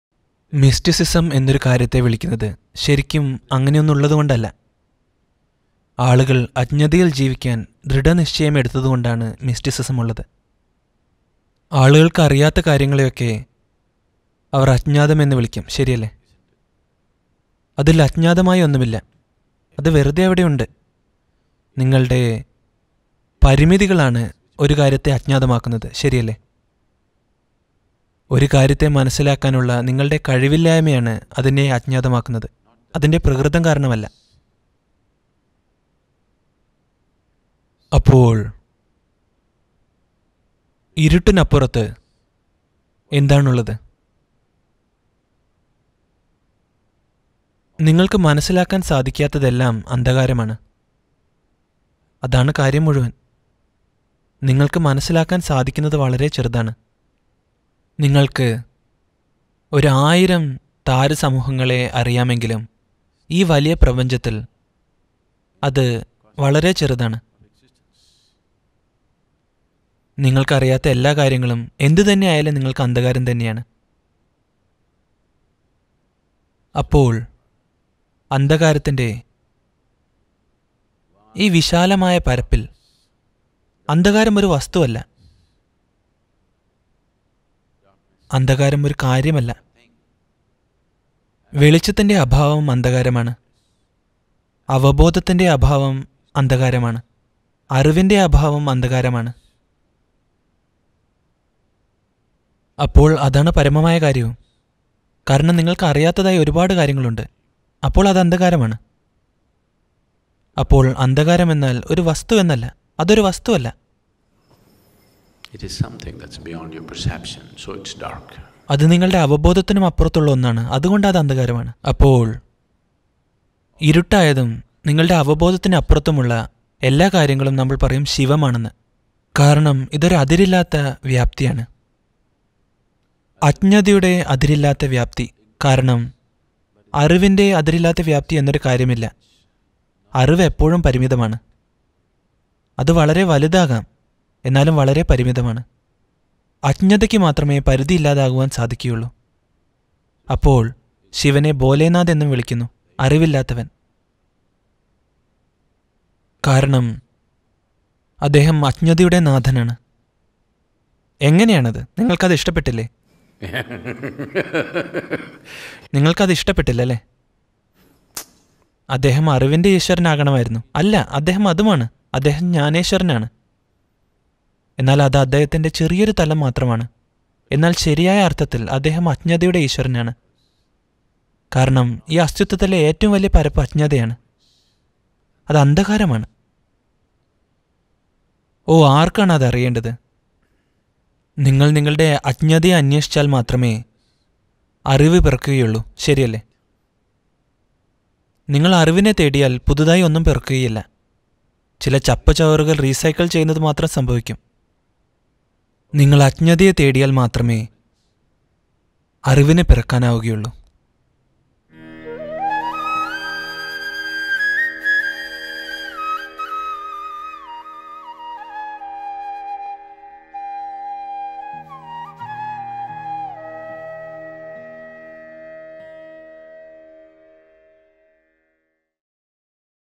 ശിവന്‍ - അജ്ഞതയുടെ ഈശ്വരന്‍ | Sadhguru Malayalam
ഈ വിശാലമായ പ്രപഞ്ചത്തില്‍, സൃഷ്ടിയുടെ ഭൂരിഭാഗവും നമ്മുടെ അവബോധത്തിനും അറിവിനും അപ്പുറത്താണ്. അജ്ഞതയുടെ ഈ അതിരില്ലാത്ത വ്യാപ്തിയെയാണ് ശിവന്‍ എന്നു പറയുന്നത്. ഒരാള്‍ തന്‍റെ അജ്ഞതയെ തേടിയാല്‍ മാത്രമേ അറിവിന്‌ പിറക്കാനാകുകയുള്ളൂ എന്നും സദ്ഗുരു പറയുന്നു.